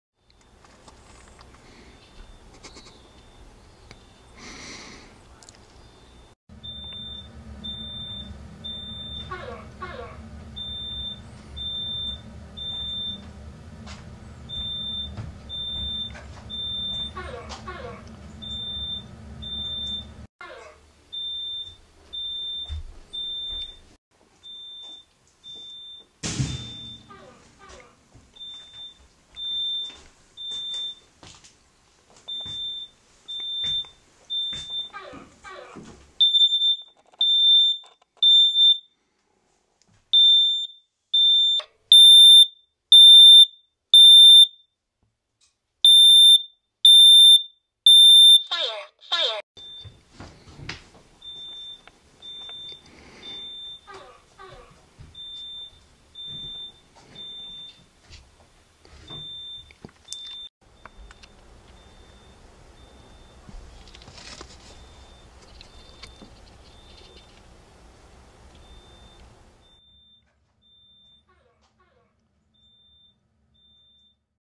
Dogs in Distress: Fire! Danger!!
The dogs are very upset when a smoke alarm went off in the middle of the night while staying at a cabin high in the Sierras...